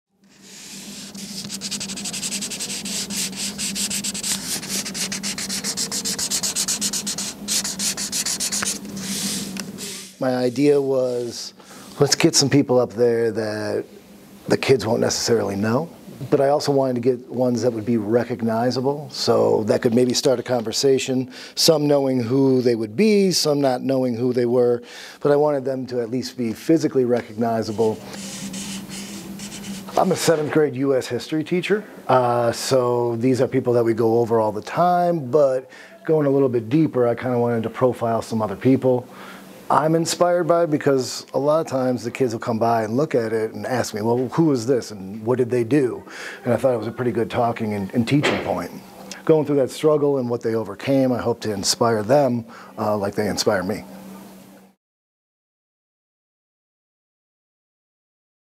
Sedway Middle School History Teacher Pays Tribute to Black History Month Through Art
Meet Mr. Korzon, the passionate history teacher at Sedway Middle School who brings learning to life through art! With a creative approach to education, Mr. Korzon pays tribute to Black History Month by showcasing a diverse array of historical figures in his captivating art displays. He inspires curiosity and sparks meaningful conversations among students, encouraging them to delve deeper into American history.